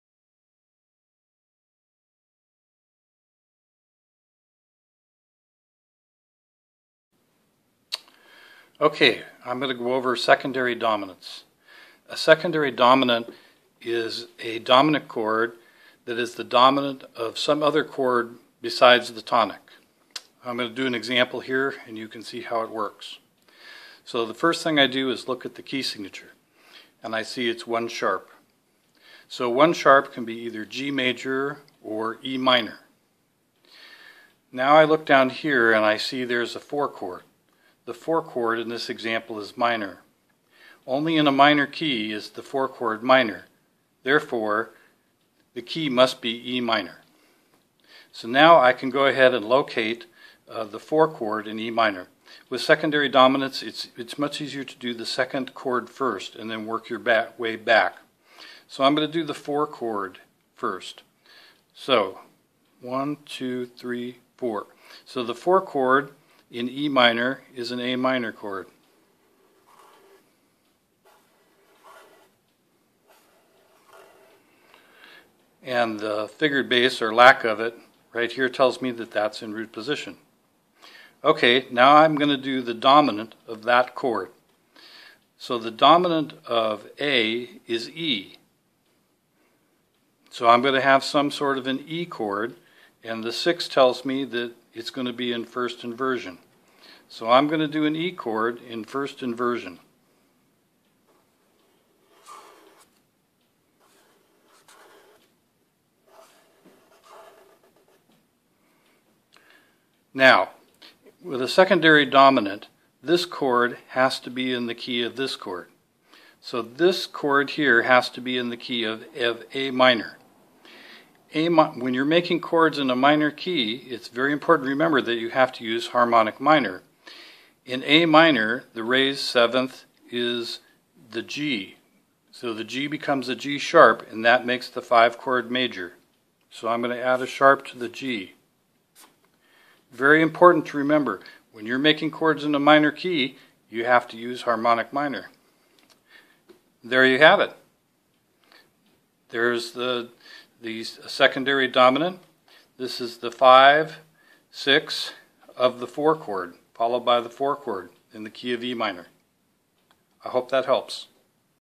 Secondary Dominants
A short tutorial on Secondary Dominants for music theory students.  I tutor music theory on Skype.